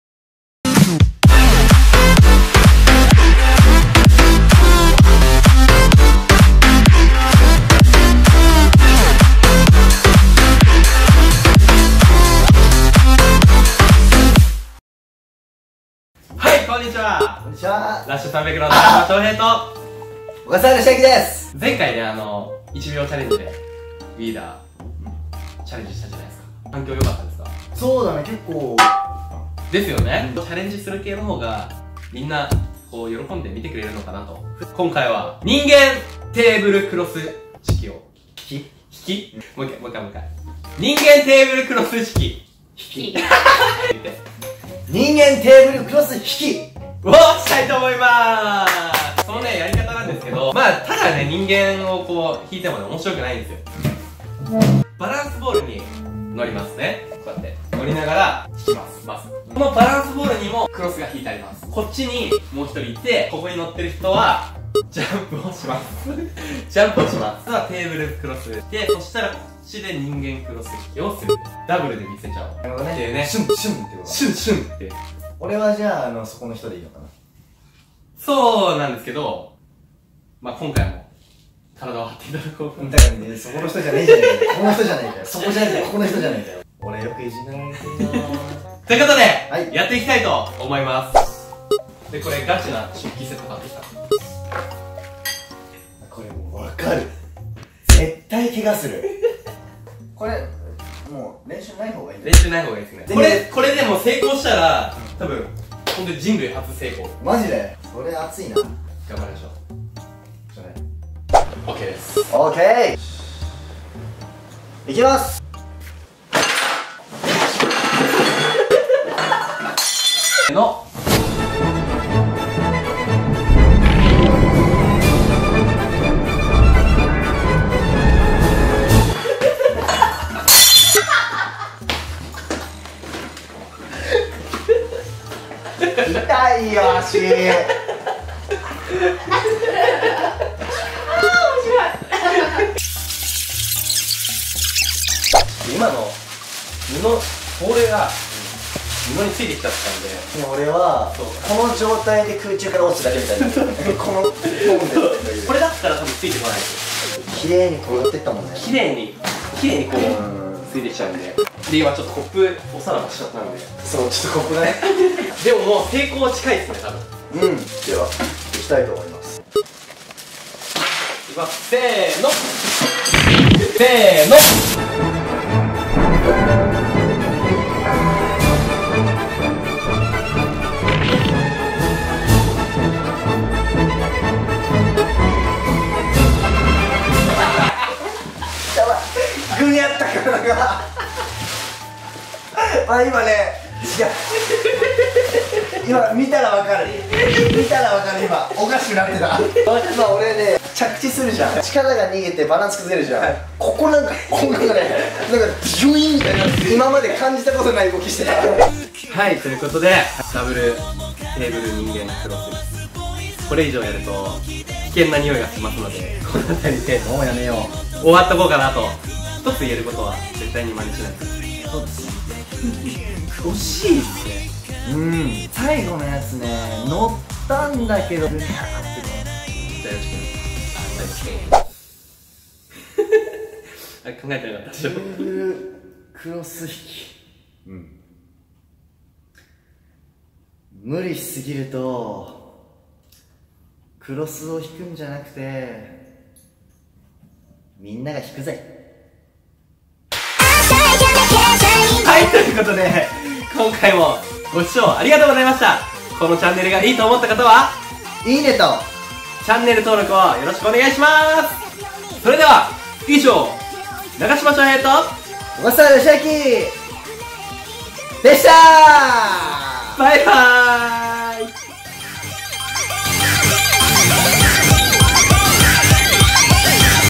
それ同時でできるか検証してみたw
バランスボールの上で人間テーブルクロス引きに挑戦！！

出演者
・NAGASHIMA SHOHEI
・OGASAWARA YOSHIAKI
・新大久保B-BOX毎週木曜日Rushナイトレギュラー出演中！

SNS
@rush_shohei   (NAGASHIMA SHOHEI)
@yoshiaki_oga  (OGASAWARA YOSHIAKI)
@rush300ex  (Rush x 300公式)

B-BOX TV Twitter : @bboxhall1